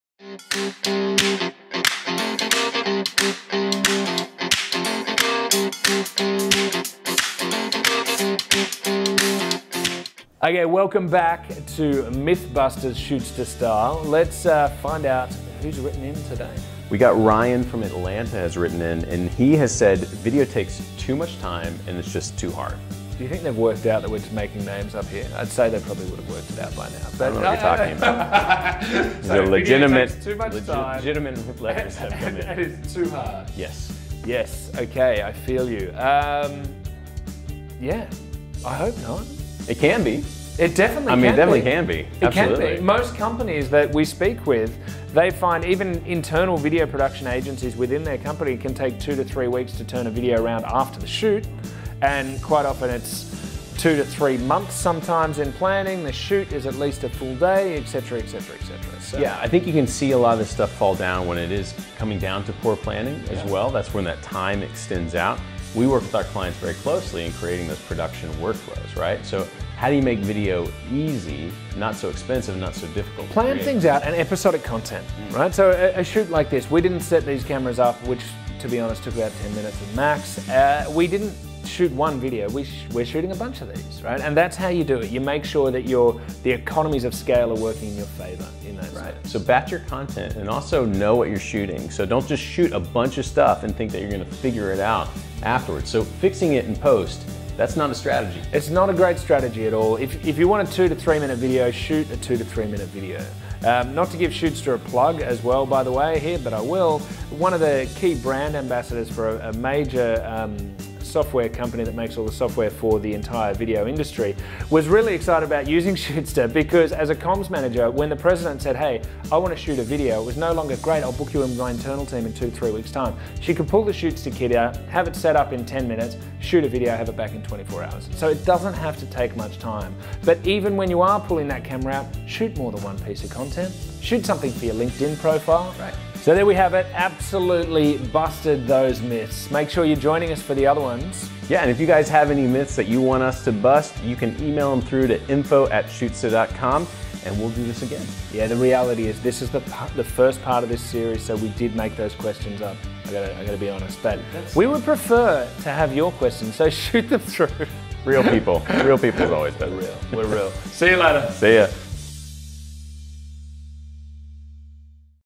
Video Mythbusters – Episode 5
Myth # 5: "Video takes too much time and is just too hard." We hope not, because video can be fun – and easier than you think!

Tune in for our latest 'Video Mythbusters', where we look at video planning, batching, and how to shoot and share your content in 24 hours (with CEO Mike Pritchett and VP of North America Chad Lakin).